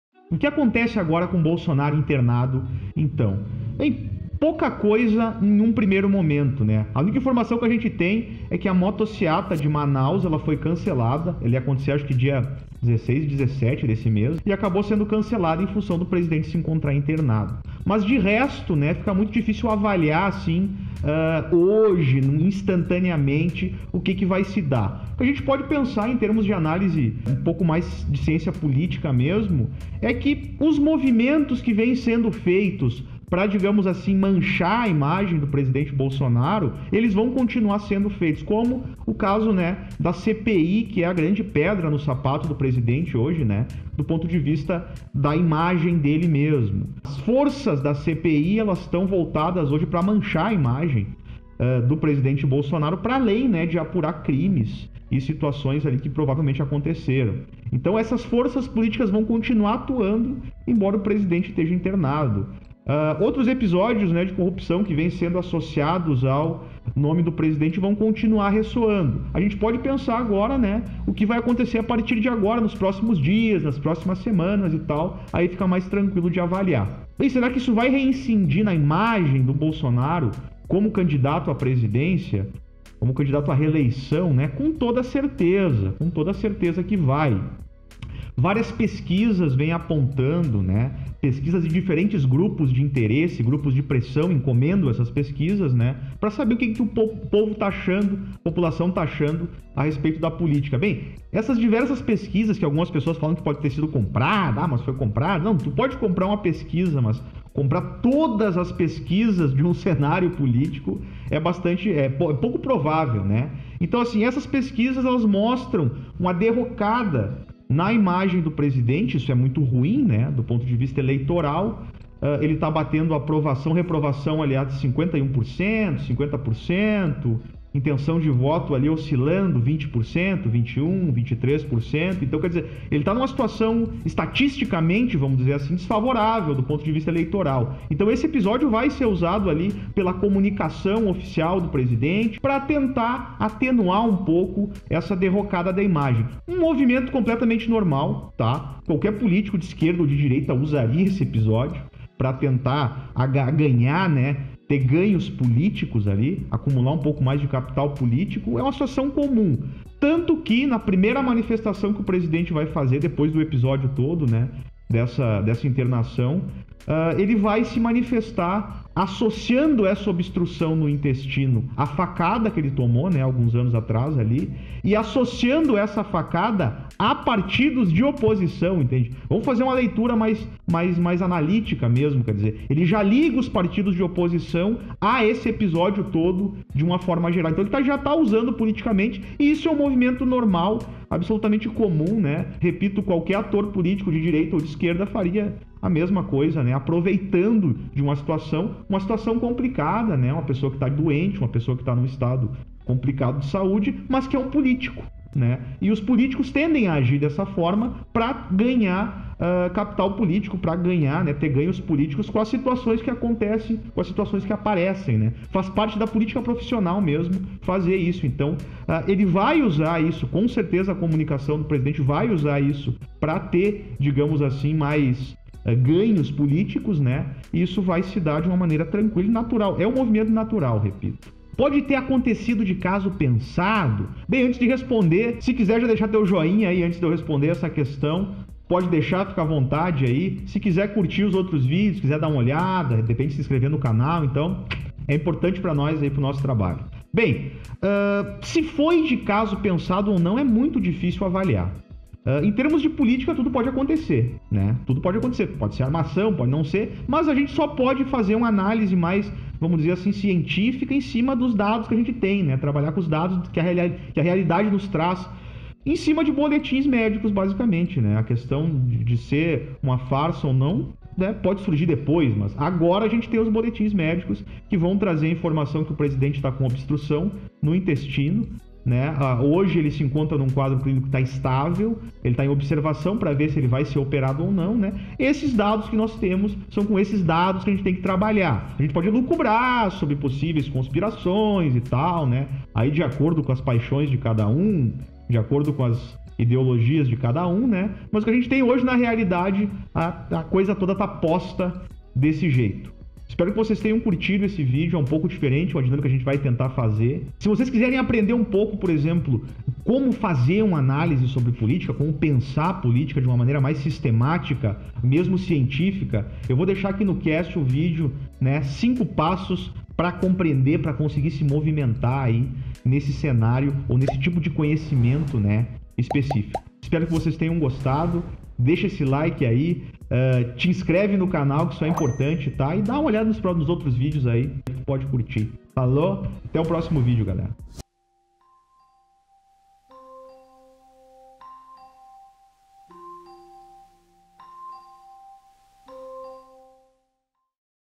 A internação de Bolsonaro: Análise Política
Bolsonaro internado: e agora???

O que acontece agora com Bolsonaro internado?

Como essa questão vai influenciar na imagem do presidente como candidato à reeleição?

Aconteceu de caso pensado?

Essas perguntas serão respondidas nesse nosso primeiro vídeo de análises sobre o cenário político brasileiro. A política tratada de forma científica.

CONTEUDO PROGRESSISTA, O TEU CANAL PRGORESSISTA NO YOUTUBE!